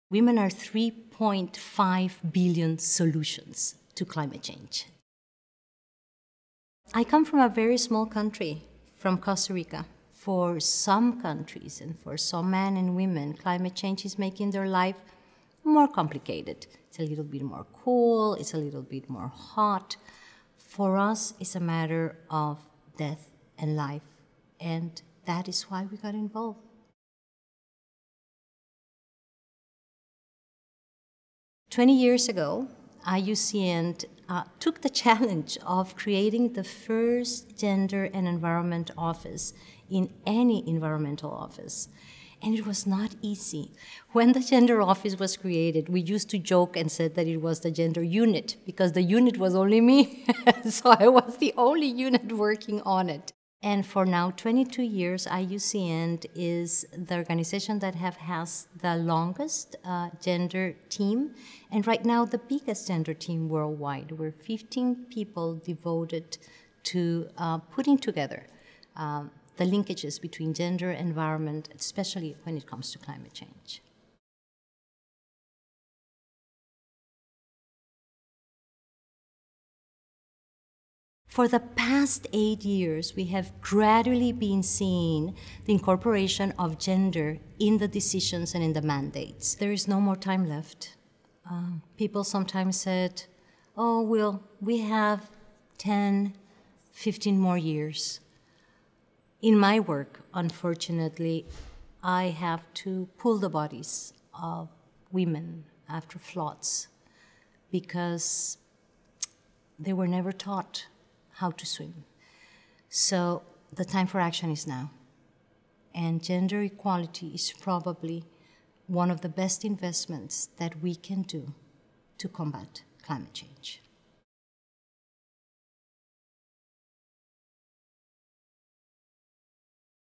Gender and Climate Expert Lorena Aguilar
Lorena Aguilar serves as a Senior Advisor for IUCN's Gender Programme. She discusses why focusing on gender is critical to developing effective, sustainable solutions to climate change.